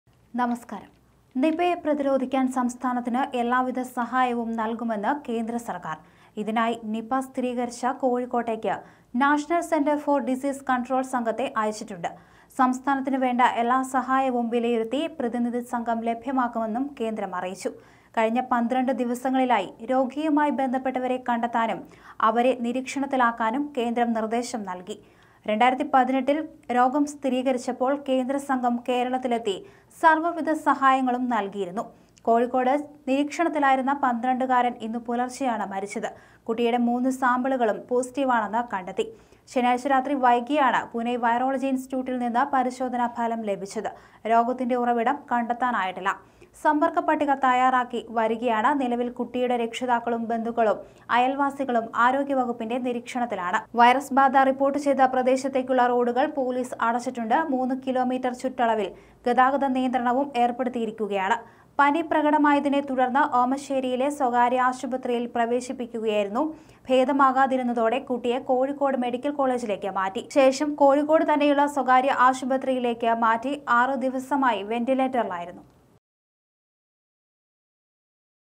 നിപയെ പ്രതിരോധം സഹായം നല്‍കി l Central government
നിപയെ പ്രതിരോധിക്കാന്‍ സംസ്ഥാനത്തിന് എല്ലാവിധ സഹായവും നല്‍കുമെന്ന് കേന്ദ്ര സര്‍ക്കാര്‍. ഇതിനായി നിപ സ്ഥിരീകരിച്ച കോഴിക്കോട്ടേക്ക് നാഷണല്‍ സെന്റര്‍ ഫോര്‍ ഡിസീസ് കണ്‍ട്രോള്‍ സംഘത്തെ അയച്ചിട്ടണ്ട്. സംസ്ഥാനത്തിന് വേണ്ട എല്ലാസഹായവും വിലയിരുത്തി പ്രതിനിധി സംഘം ലഭ്യമാക്കുമെന്നും കേന്ദ്രം അറിയിച്ചു.